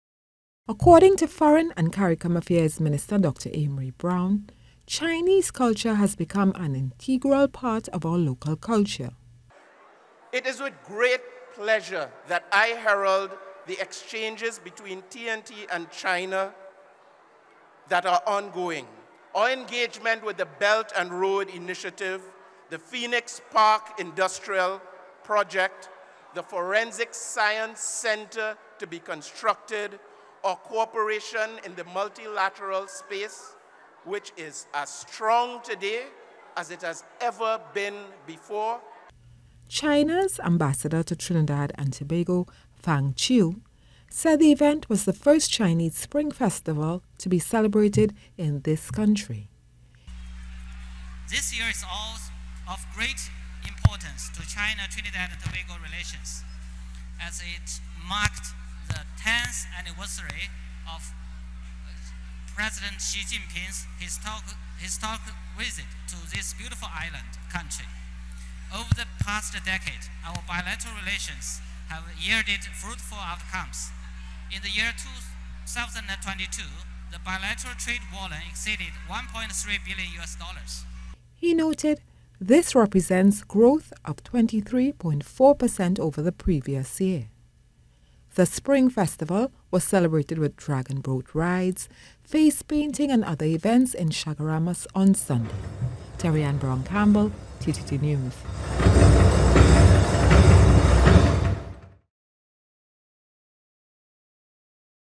First Chinese Spring Festival Celebrated In T&T
Ties between this country and the People's Republic of China continue to be both economically and culturally strong, according to Minister of Foreign & CARICOM Affairs, Dr. Amery Browne.

Cameraman Andy Wharton attended the Chinese Spring Festival launch with reporting by Terry-Ann Browne-Campbell.